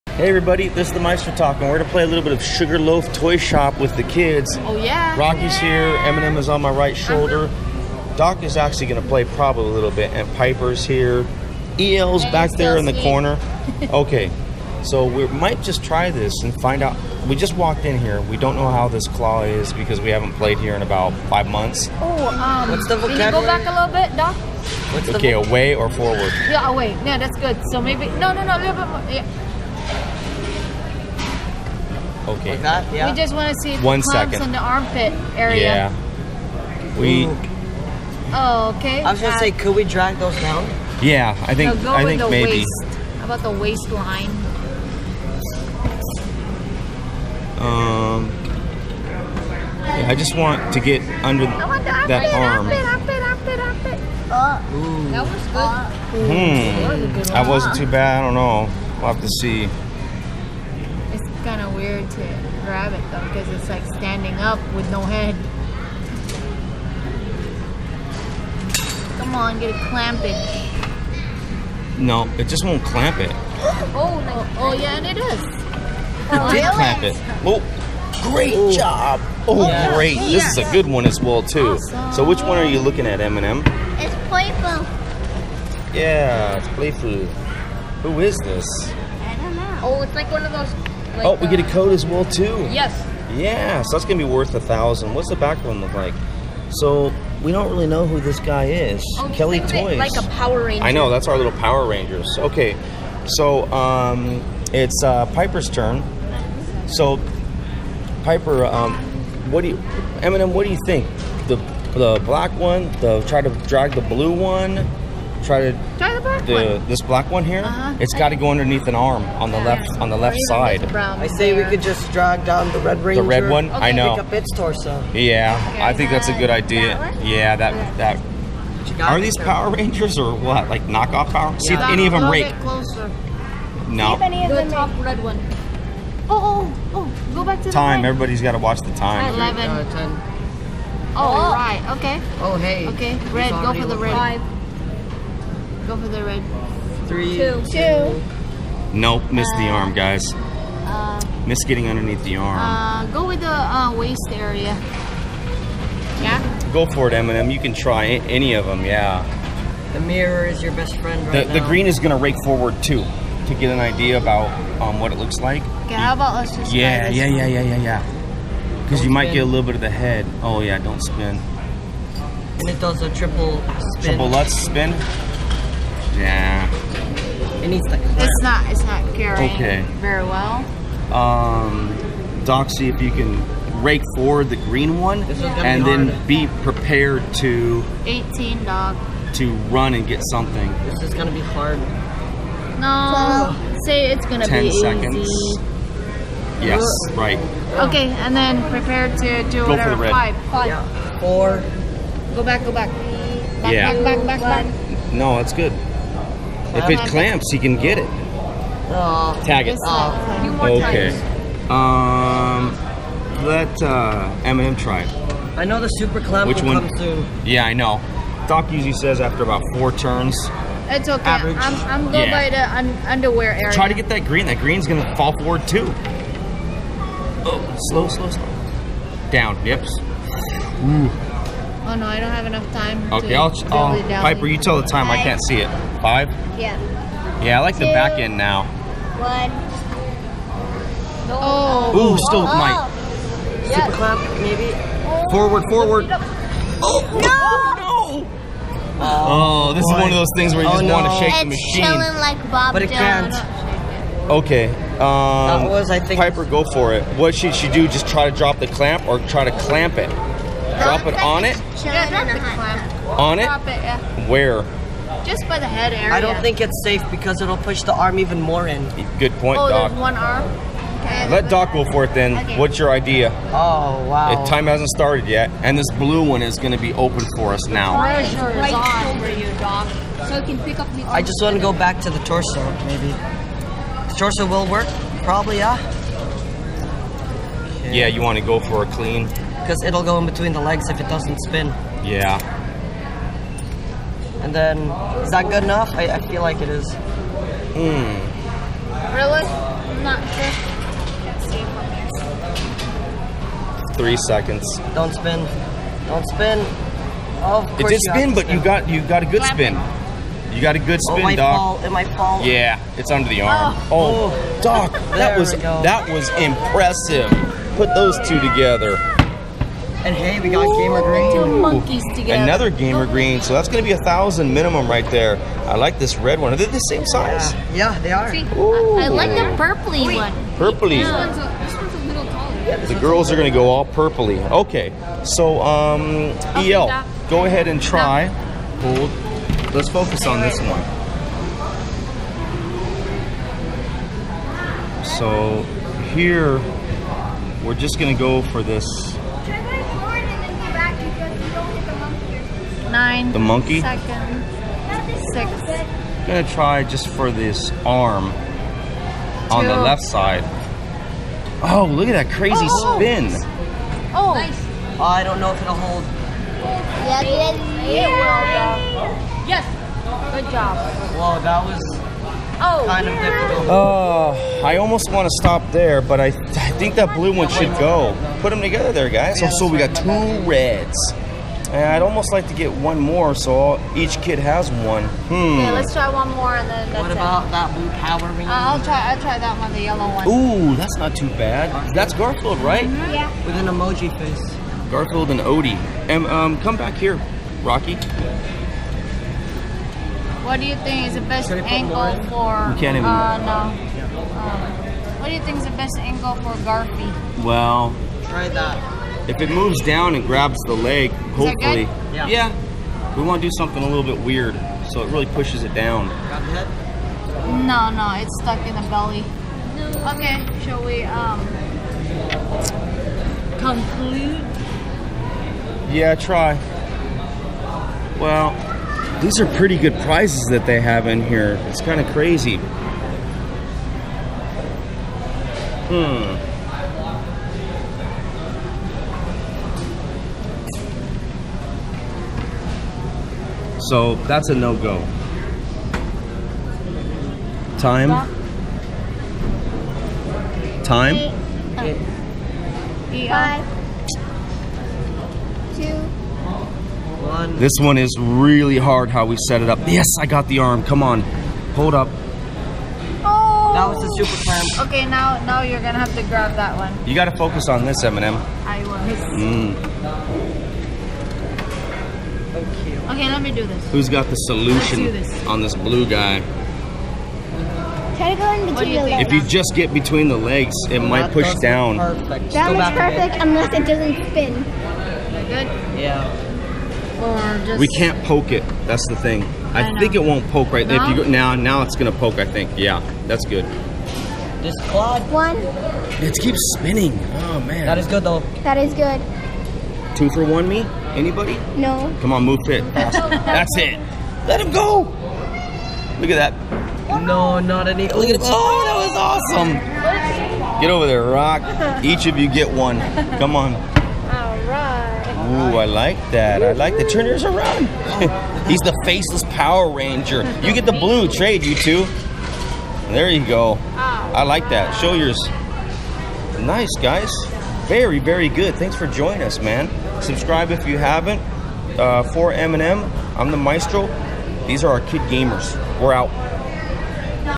Claw Machine Power Ranger Toy Prize Wins Playing Skill Crane Game: Doc, E.L., Rocky, Piper, M&M
Claw Machine Power Ranger Toy Prize Wins Playing Skill Crane Game kids challenge. アメリカのゲームセンター クレーンゲーム 子供の楽しさ.

Arcade Prize Wins Playing The Claw Machine Skill Crane with Doc, E.L., Rocky & Piper with Crane Game Strategists The Maestro & M&M. We went a bit bonkers today trying to win some of these Power Ranger look-alike Plushie Toys made by Kelly Toys at the Amusement Center today. And it ended up being well worth it too! The Kids were happy they all were able to win one Power Ranger Toy each to take home and use to throw at each other!

CLAW MACHINES PLAYED IN THIS VIDEO?
Sugar Loaf Toy Shoppe Claw Machine Skill Crane (Recommended By All Of Our Gamer Team)